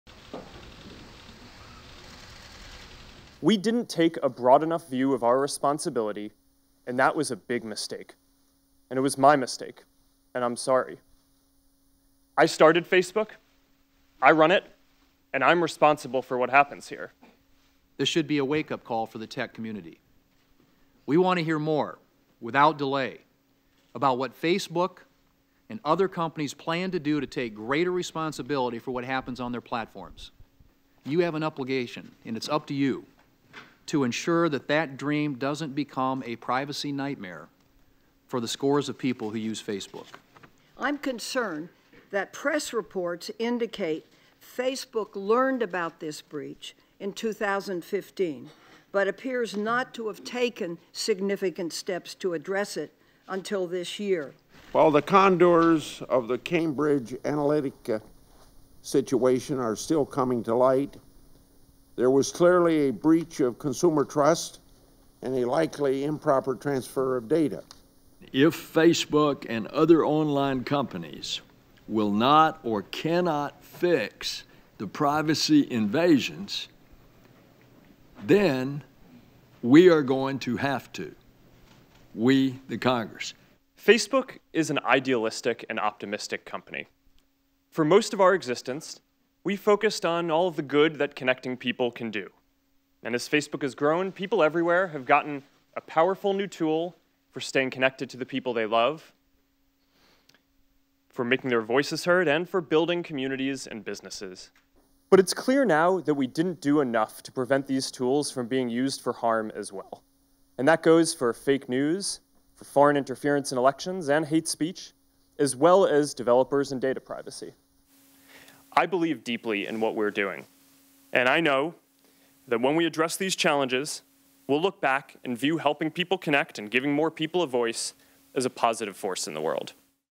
Zuckerberg apologizes as senators question Facebook's data practices
Facebook CEO Mark Zuckerberg sat down before lawmakers on April 10, and apologized, answered questions and defended the tech giant amid controversies over data privacy.